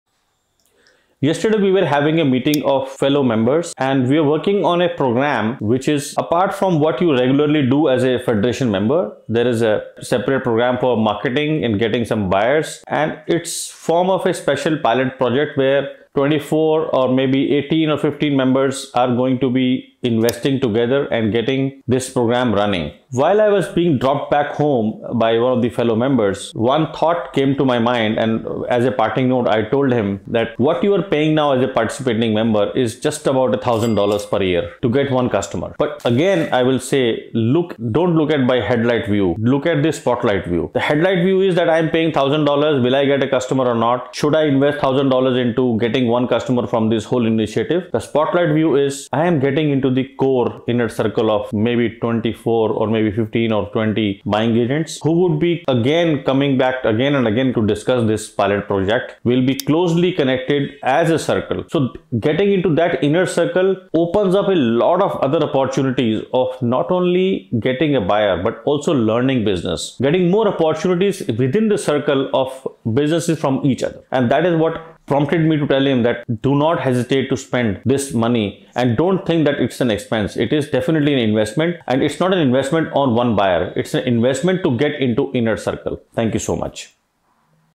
The FBA Pilot Program: A game-changer for your business
In this video, we want to share with you an exciting program that we've been working on as members of our Federation. This program is separate from our regular activities and focuses on marketing and attracting buyers through a special pilot project.

The idea behind this project is to bring together a group of 24, 18, or 15 members who will invest together to make this program a success. Recently, while I was being dropped back home by one of our fellow members, a thought occurred to me. I told him that when you look at the cost of being a participating member, which is around $1,000 per year, it may seem like a lot to pay for just one customer.

However, I urged him not to view it from a narrow perspective. Instead, I encouraged him to look at it from a broader standpoint, like a spotlight illuminating the true benefits. The headlight view focuses on the immediate cost of a thousand dollars and questions whether it's worth investing to acquire a single customer through this initiative.

But the spotlight view is different. By participating in this program, we gain access to an exclusive inner circle of 24, 15, or 20 buying agents. This circle will engage in ongoing discussions about the pilot project and remain closely connected. Getting into this inner circle presents numerous opportunities beyond just acquiring buyers. We can learn from each other, expand our business knowledge, and discover new opportunities within the circle.

This realization led me to tell my fellow member that he shouldn't hesitate to invest in this program. It's not merely an expense; it's a strategic investment to gain entry into this valuable inner circle. So, I want to emphasize that spending this money is worthwhile and holds immense potential for growth.